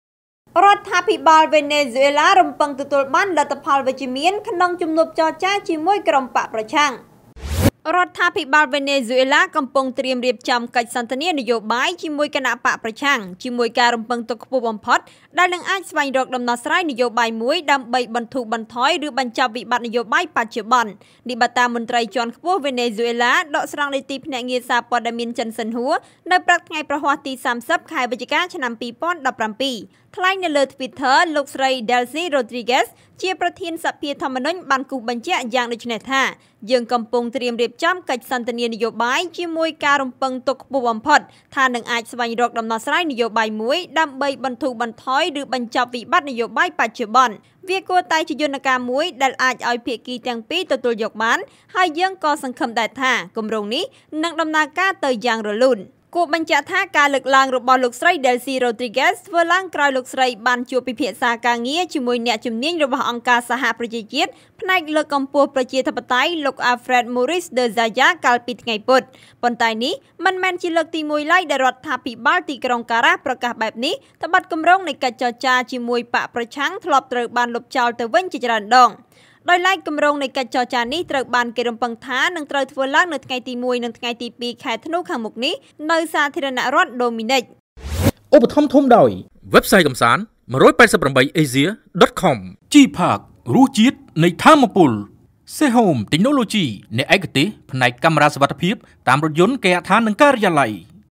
គួរឲ្យចាប់អារម្មណ៍! រដ្ឋាភិបាលវេណេស៊ុយអេឡា «រំពឹងទទួលបាន លទ្ធផលវិជ្ជមាន...
គួរឲ្យចាប់អារម្មណ៍! រដ្ឋាភិបាលវេណេស៊ុយអេឡា «រំពឹងទទួលបាន លទ្ធផលវិជ្ជមាន» ក្នុងជំនួបចរចាជាមួយ​ក្រុមបក្សប្រឆាំង... (រាយការណ៍ព័ត៌មានដោយកញ្ញា សំ ស្រីនីត, Fresh News TV)